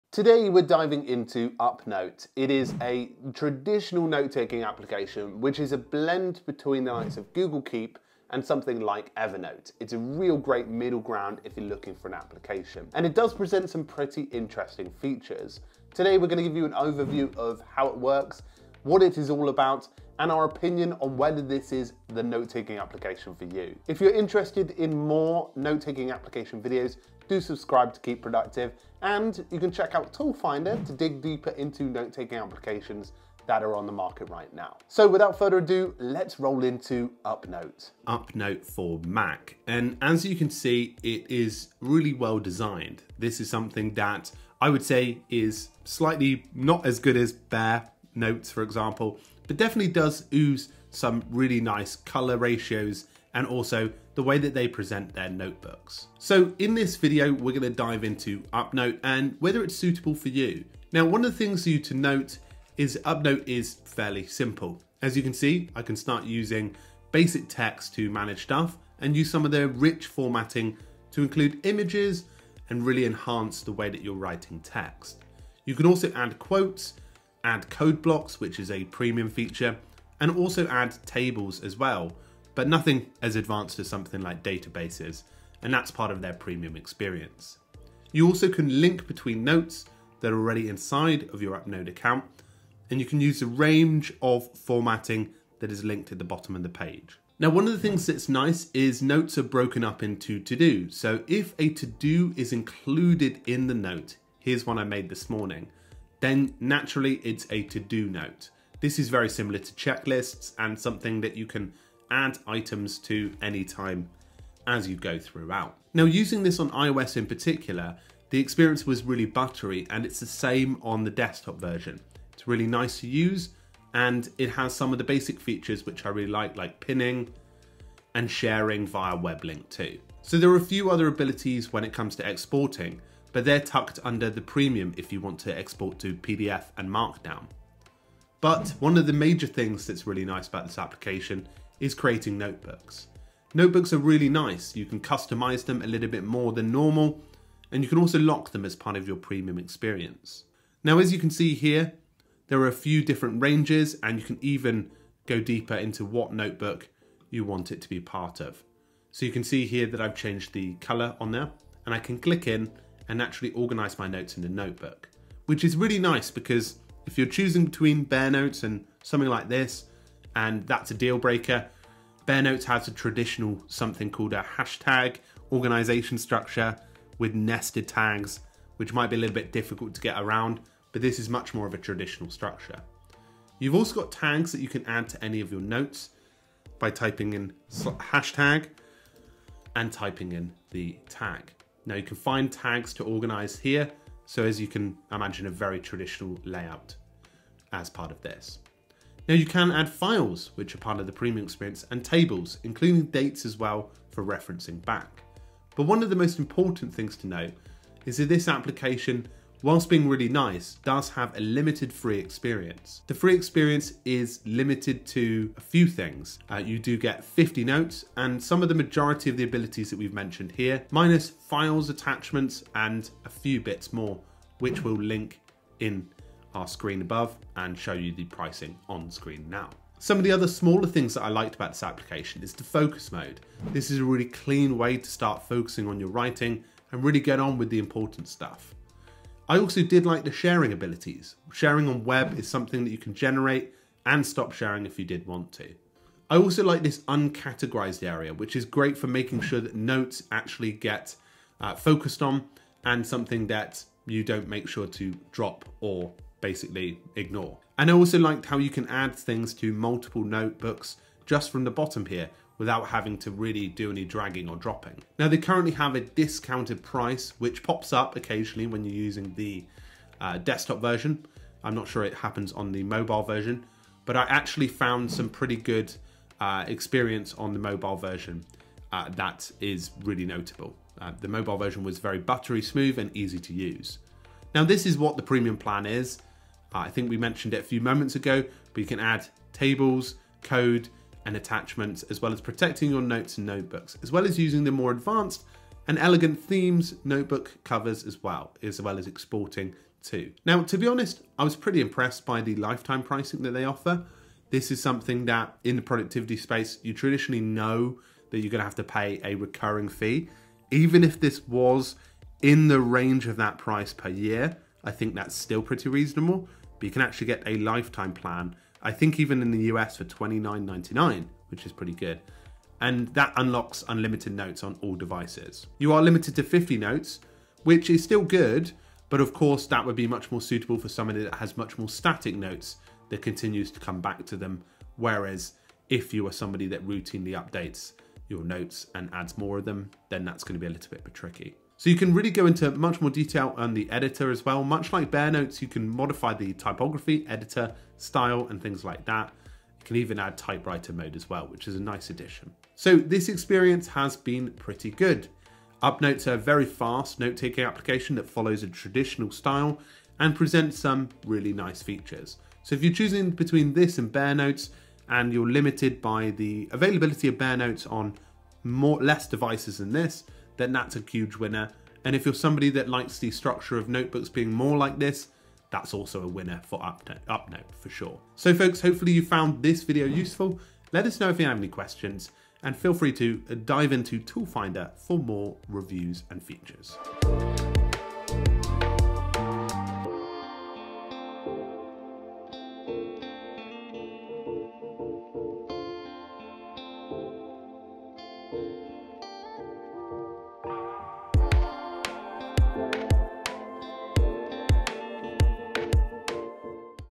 Upnote for Notes: A Lightweight Masterpiece
Stop doing busywork! Try Bento Focus Find the best productivity tools with our Upnote is a surprisingly snappy notes app with a fast app, wide availability and decent features for a note-taking application. Today, we summarise Upnote and what it offers. Explore it further on Tool Finder

⏰ TIMESTAMPS

00:00 - Introduction
00:52 - What is Upnote?
03:44 - Pricing
03:59 - Features I Love
05:02 - Summary